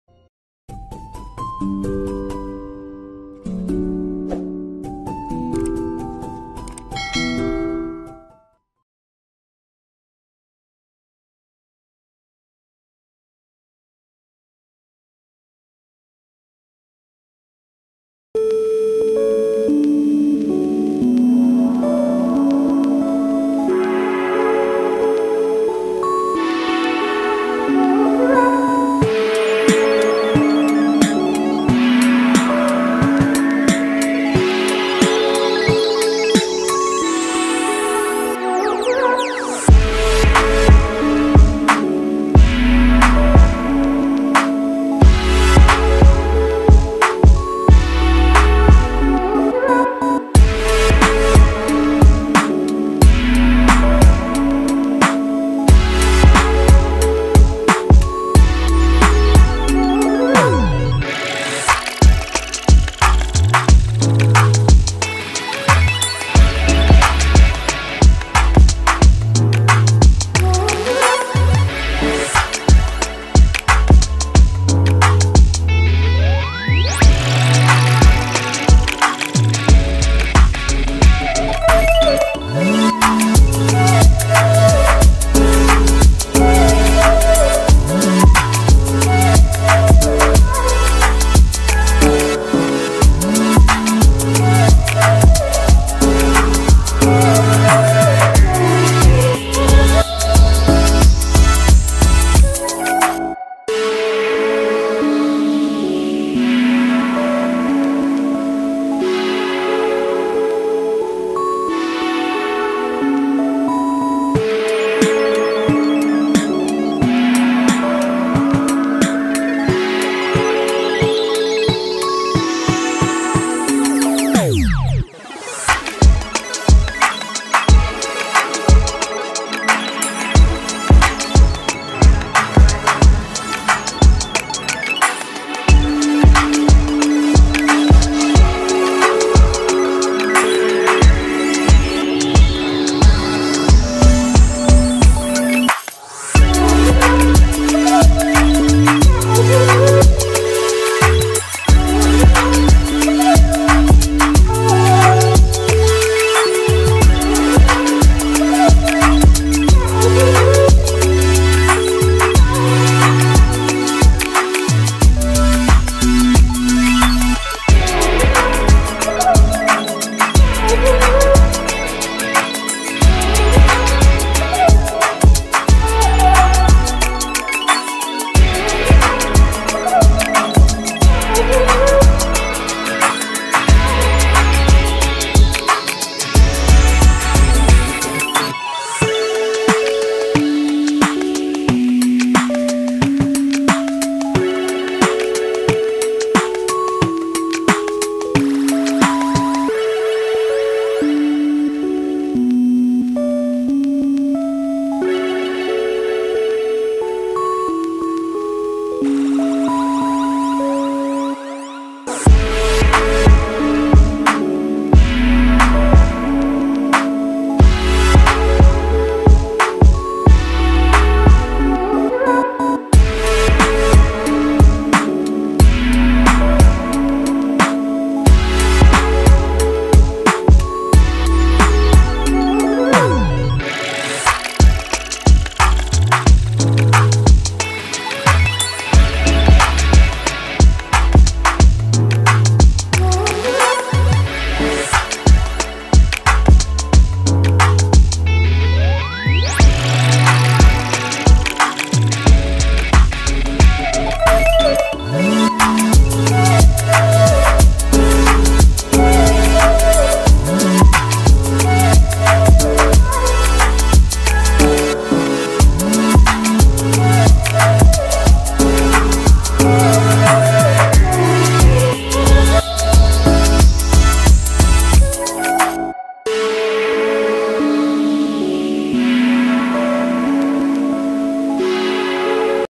Review of WB by Hemani Products | Unboxing Sunscreen and Porefiner|Review & prices | Lovleena Fairy
Review of WB by Hemani Products | Unboxing Sunscreen and Porefiner | Review And prices of Products